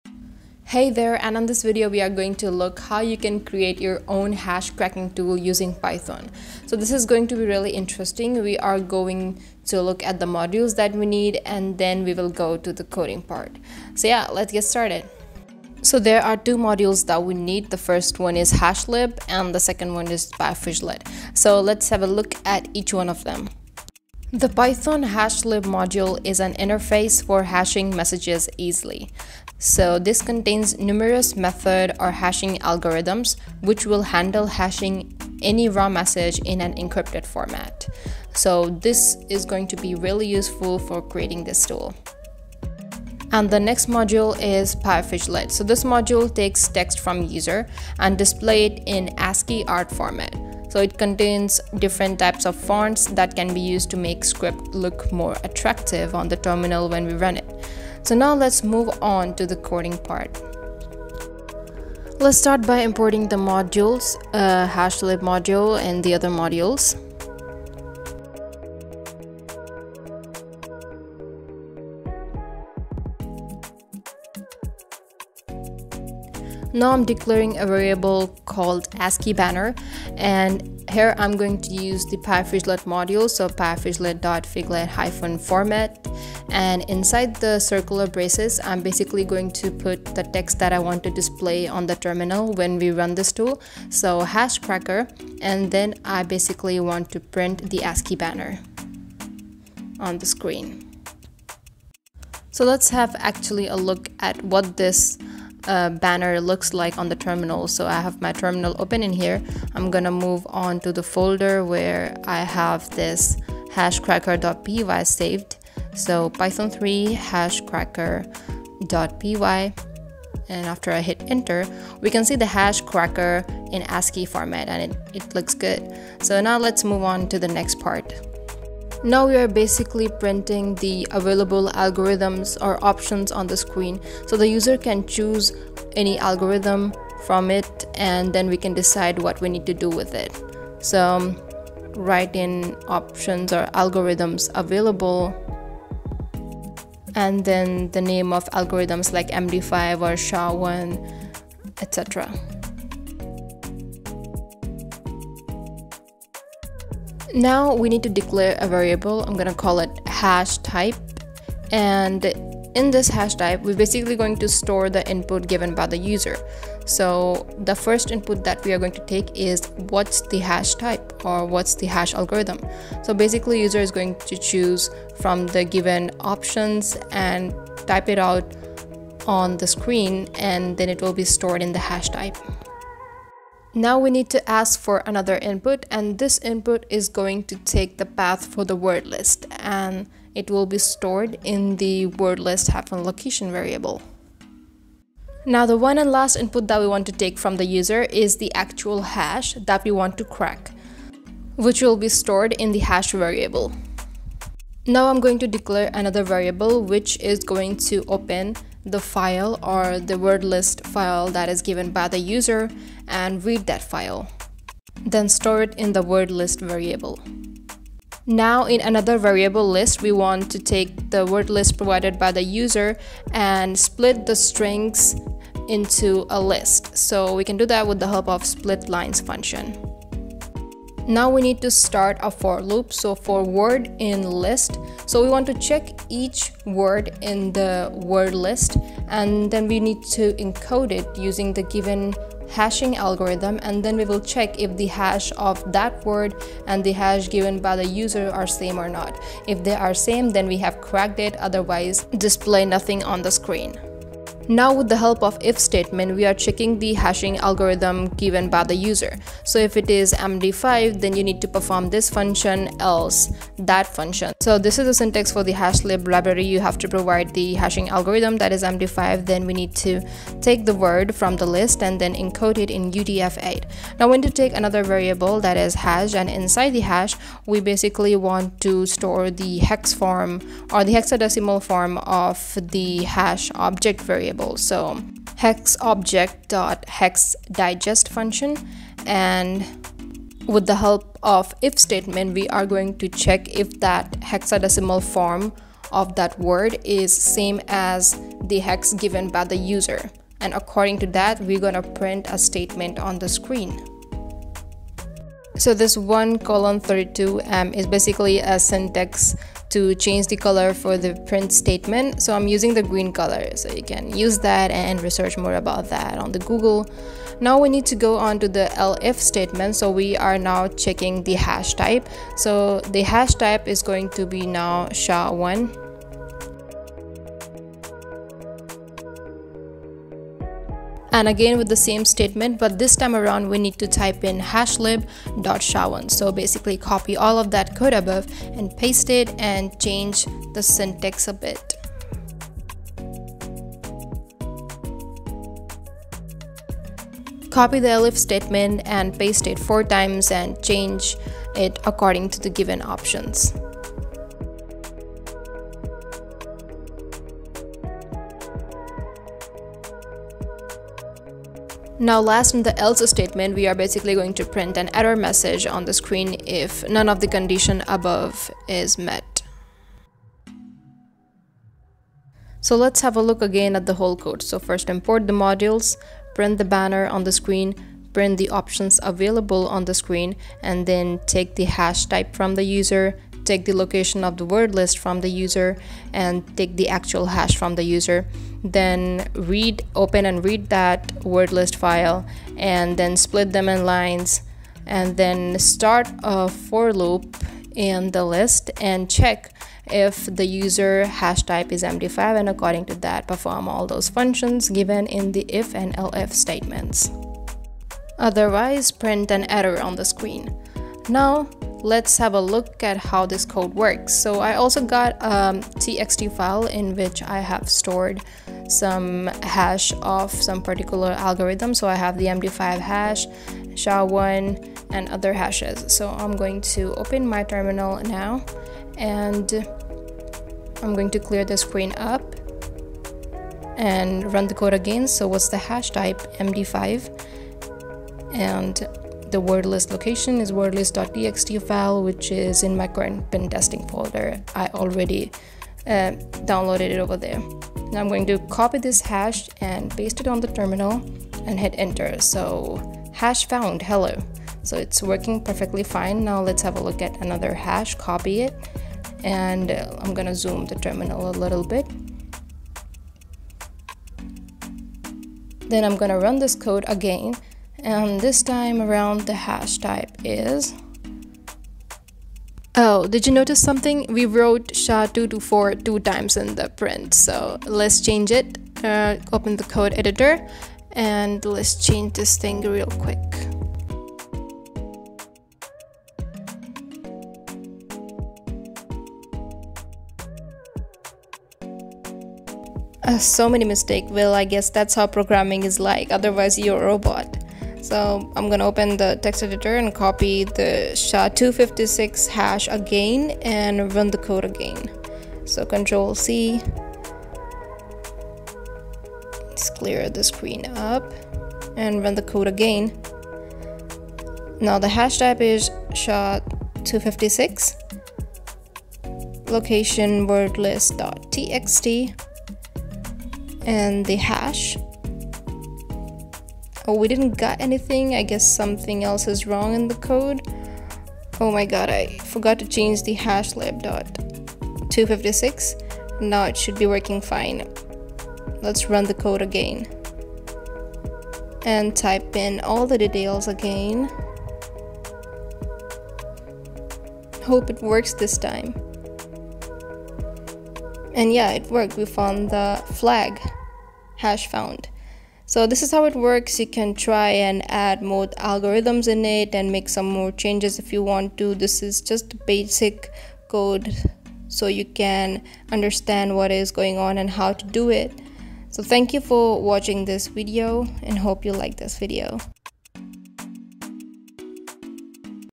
Create your Own Hash Cracking Tool Using Python | With Slides | Explained
Hey, this video will teach you how you can create your own Hash Cracking tool using Python.
It is explained step by step with the help of slides and is beginner-friendly.